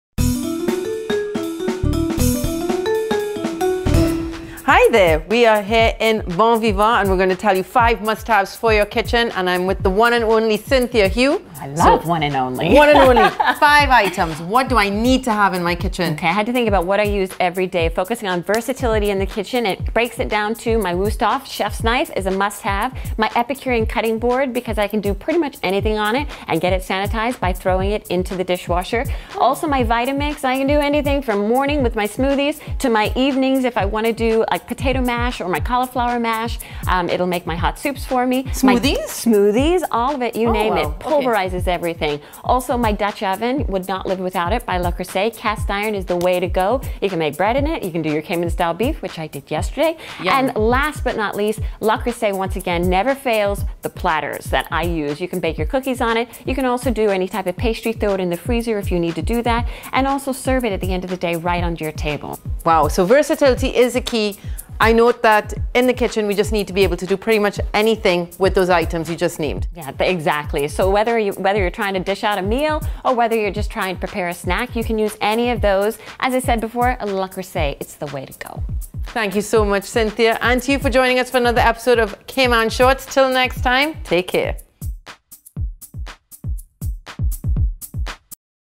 Five must haves in your Grand Cayman kitchen
If you are living in Grand Cayman, you need to make sure you have the essentials at your finger tips when it comes to preparing a meal for your family and friends. There is no shortage of visitors when you live in paradise!

In this episode of Cayman Shorts, we head to Bon Vivant at Camana Bay to find out what the five must haves are for your Cayman Islands kitchen.

Cynthia Hew of Bon Vivant joins Tammi to give us 5 versatile items we need to have in our kitchen:
1. Wusthof Chef’s Knife
2. Epicurean Cutting Board – Because you can do pretty much anything on it and can even sanitize it in the dishwasher.
3. Vitamix Blender – From mornings to evenings, you can blend smoothies or even potato or cauliflower mash and soups. It pulverizes everything.
4. Dutch Oven – Cast iron is the way to go. You can bake bread in it or even cook Cayman style beef.
5. Le Creuset Platters – You can bake cookies on it or any type of pastry, place it in the freezer and serve it at the end of the day on your table.
Whether you’re trying to dish out a meal or prepare a snack, you can use any of the items to make the perfect meal.